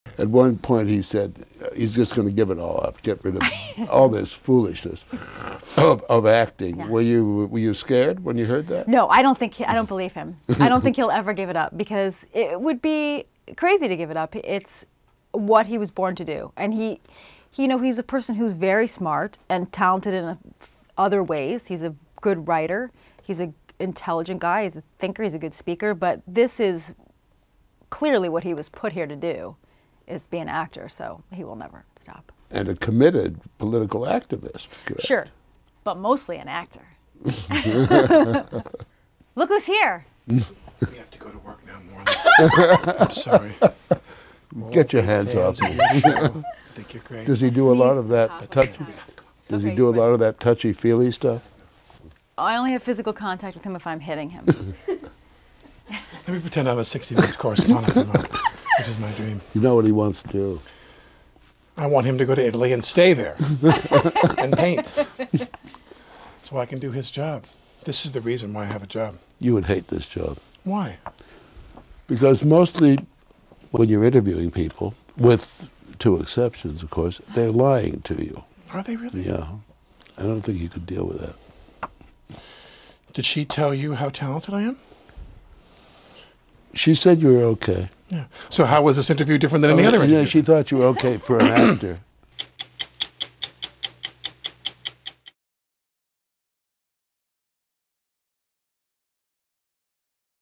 Web Extra: Outtakes
Alec Baldwin interrupts Tina Fey's interview and reveals his dream job.